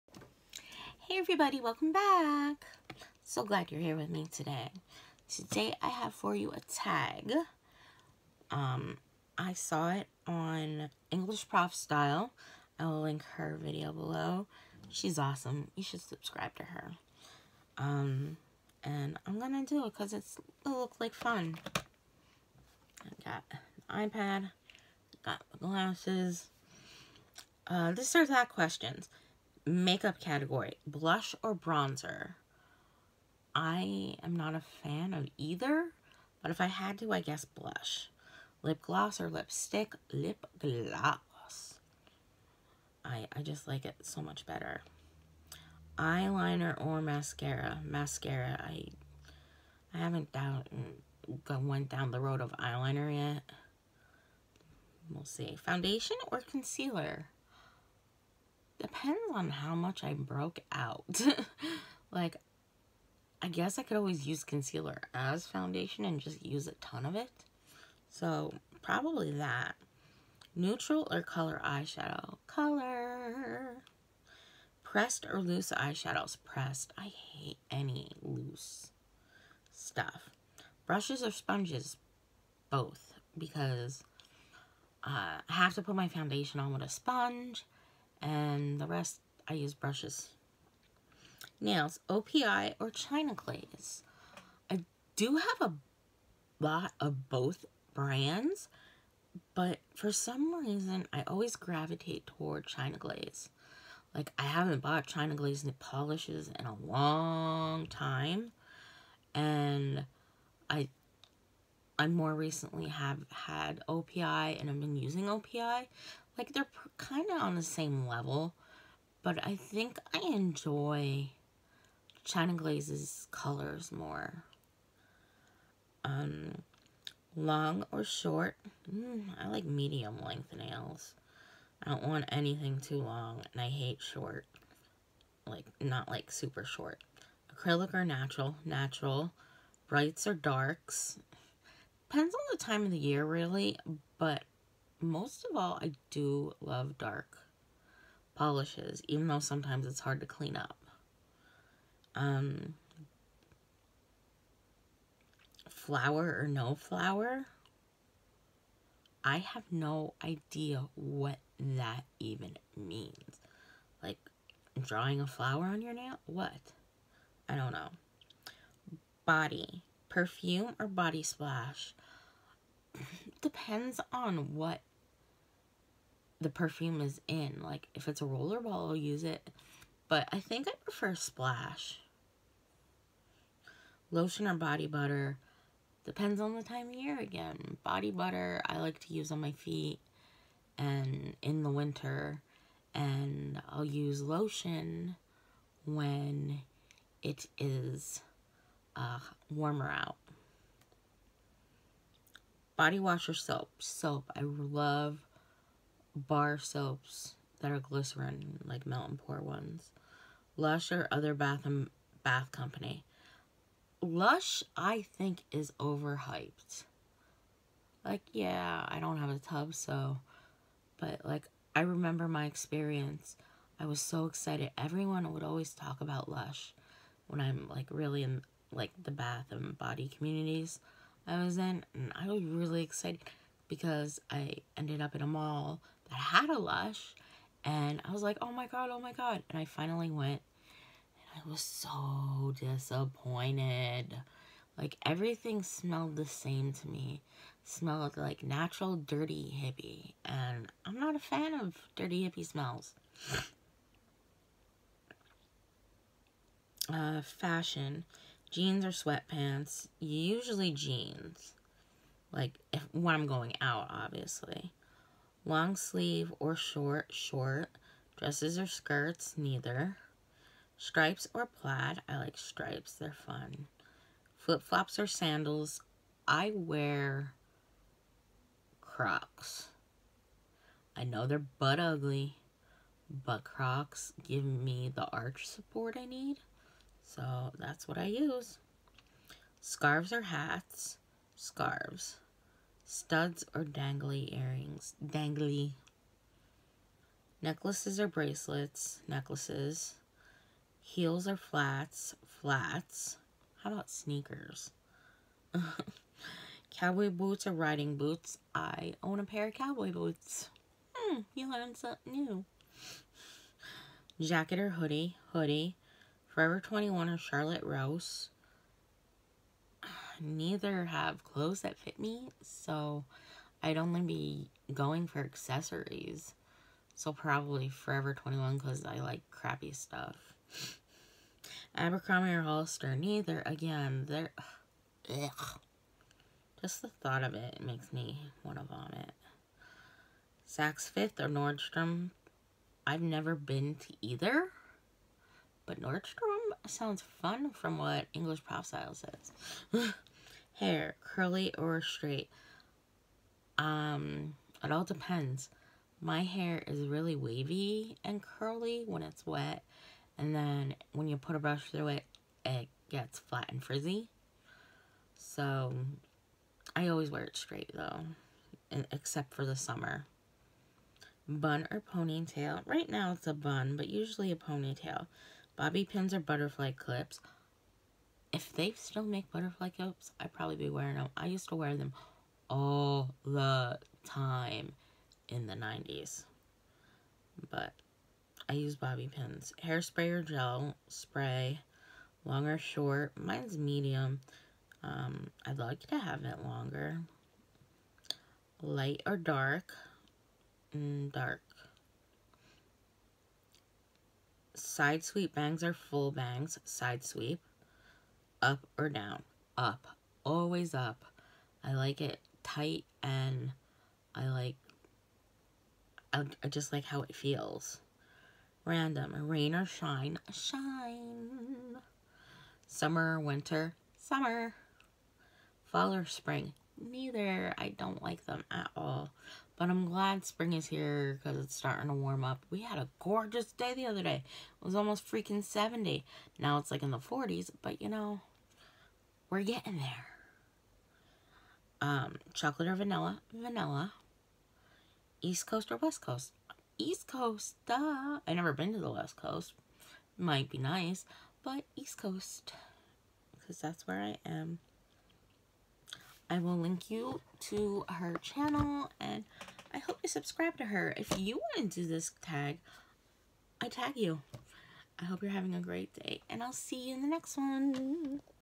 This Or That Makeup Tag
This or That Questions:
→ Makeup
Blush or Bronzer
Lipgloss or Lipstick
Eyeliner or Mascara
Foundation or Concealer
Neutral or Color Eyeshadow
Pressed or Loose eye shadows
Brushes or Sponges

→ Nails
O.P.I or China Glaze
Long or Short
Acrylic or Natural
Brights or Darks
Flower or No Flower

→ Body
Perfume or Body Splash
Lotion or Body Butter
Body wash or Soap
LUSH or Other Bath Company

→ Fashion
Jeans or Sweat pants
Long sleeve or Short
Dresses or Skirts
Stripes or Plaid
Flip flops or Sandals
Scarves or Hats
Studs or Dangly Earrings
Necklaces or Bracelets
Heels or Flats
Cowboy Boots or Riding Boots
Jacket or Hoodie
Forever 21 or Charlotte Russe
Abercombie or Hollister
Saks 5th or Nordstrom

→ Hair
Curly or Straight
Bun or Ponytail
Bobby pins or Butterfly clips
Hair spray or Gel
Long or Short
Light or Dark
Side sweep bangs or Full bangs
Up or Down

→ Random
Rain or Shine
Summer or Winter
Fall or Spring
Chocolate or Vanilla
East coast or West coast

Sign up and buy from Bomb Diggy with this link!